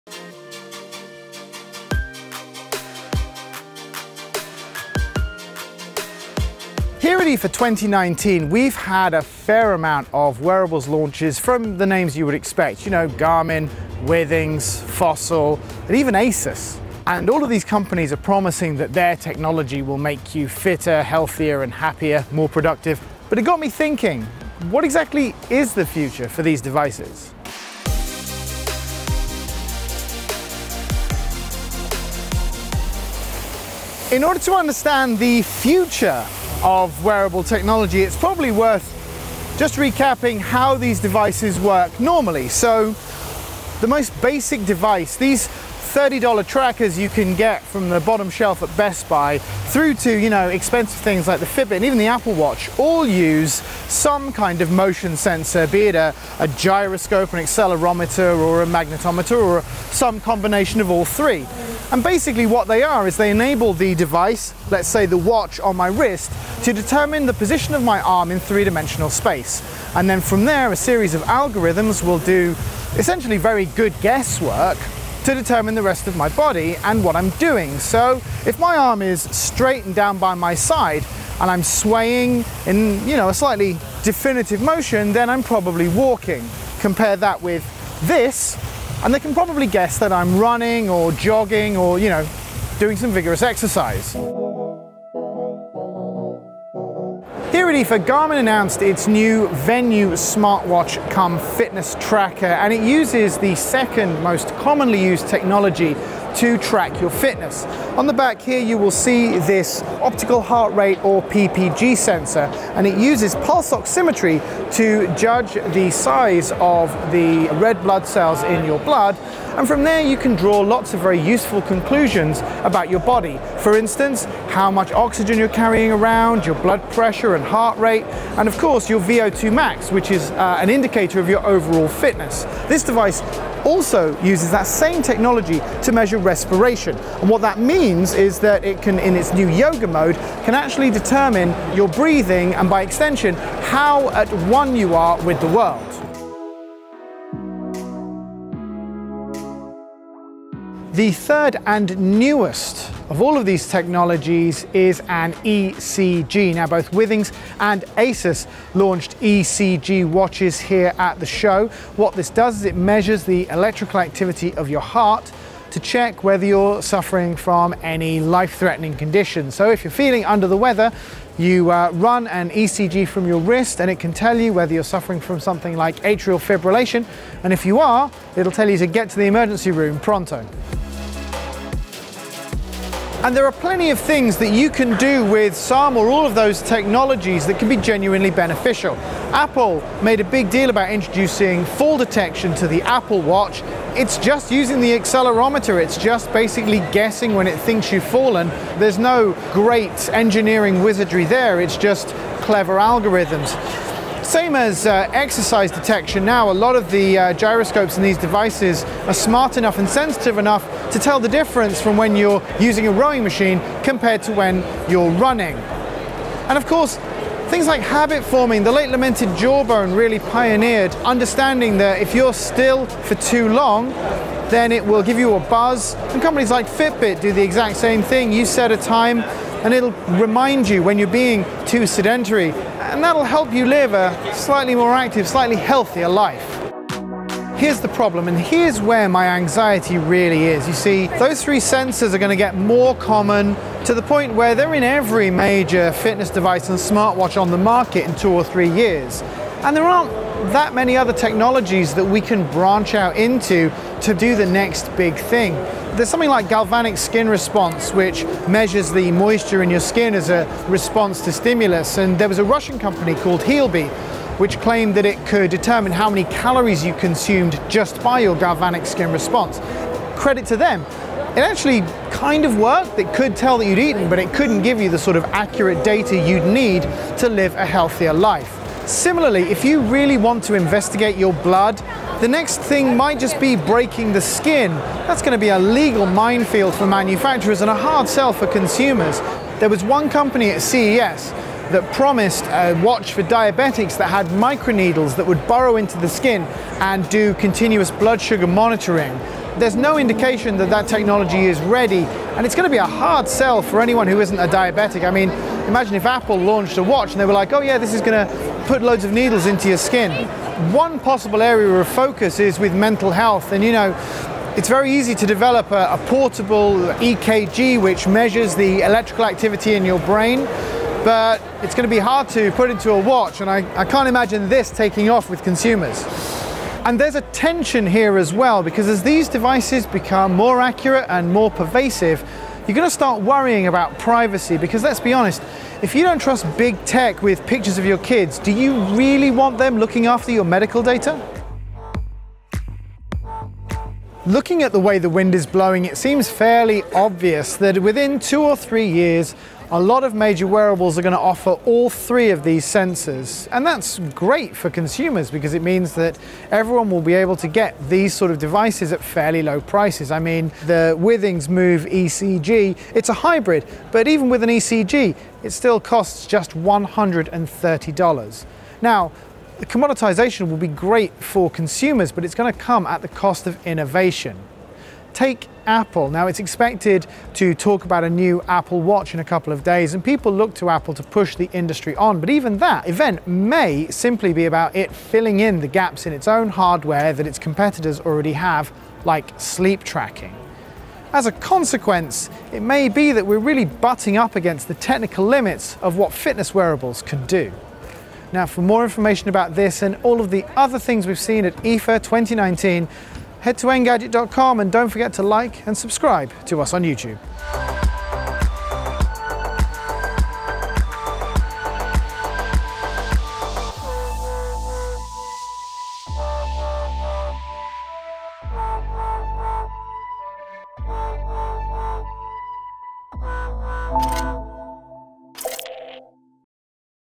What is the future of fitness wearables?
Are wearables still advancing or have they hit a wall?